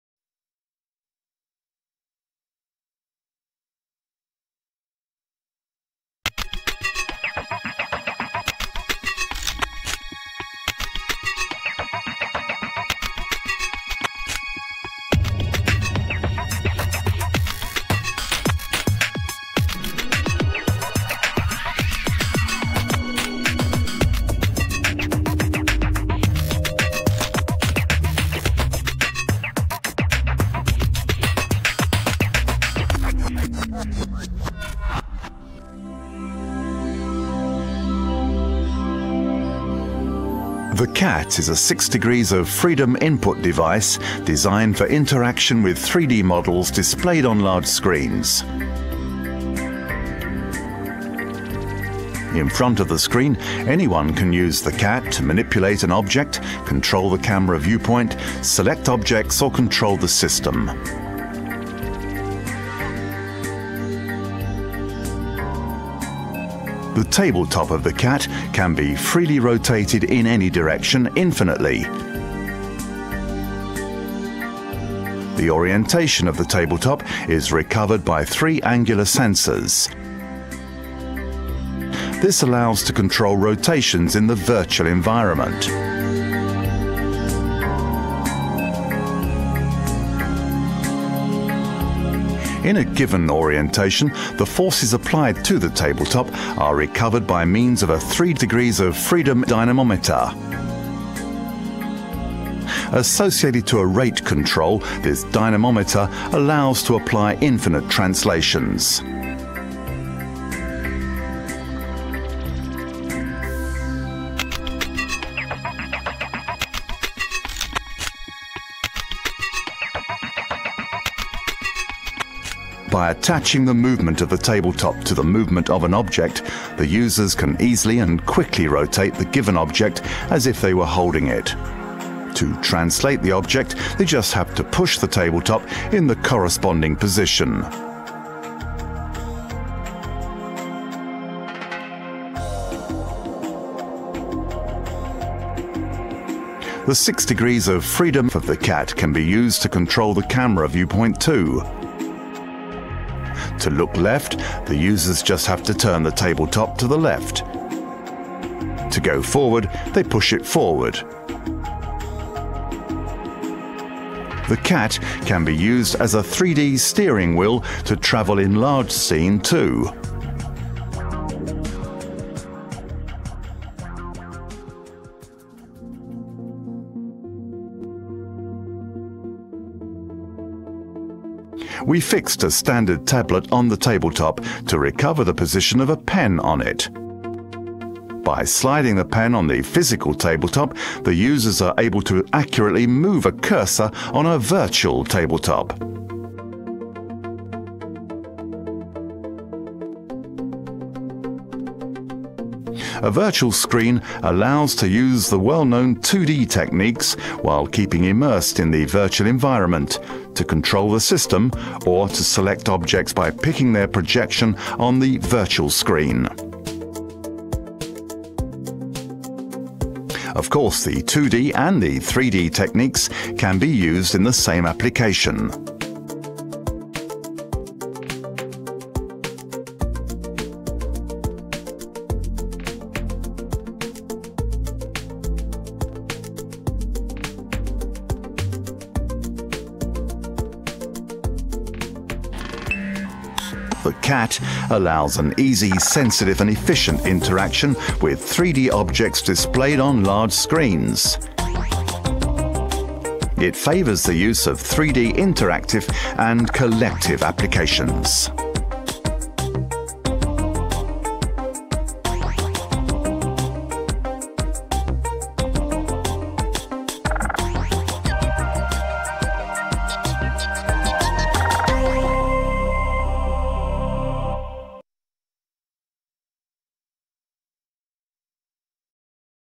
The CAT - Control Action Table
The CAT is a 6DOF input device designed for interaction with 3D environments displayed on large screens